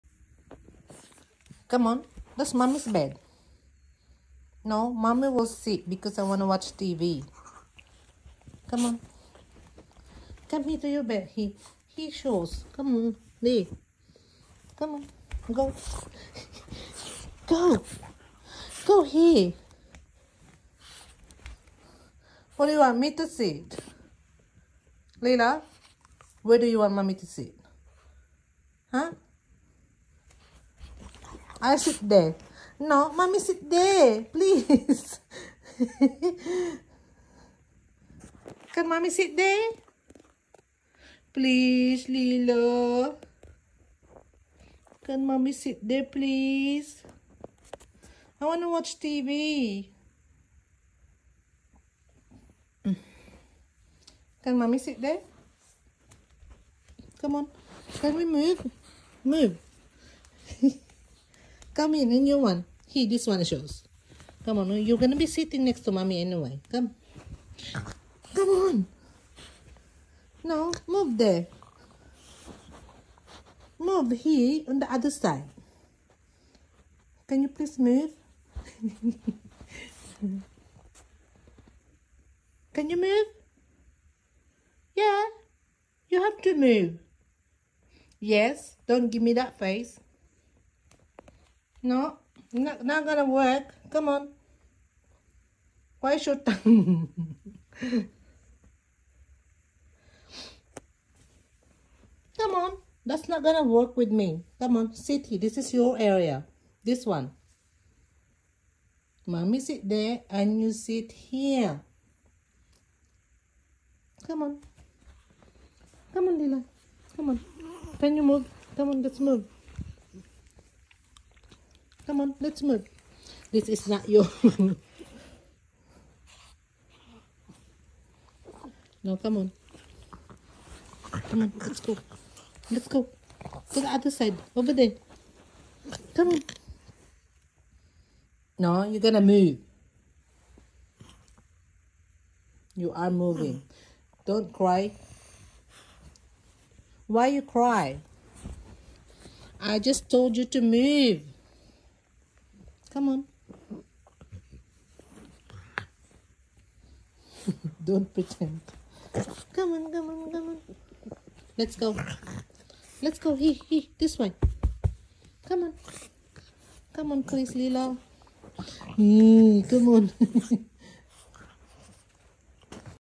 Leela the Frenchie cried!
When Leela dog refused to move and cried at the end🙈🙈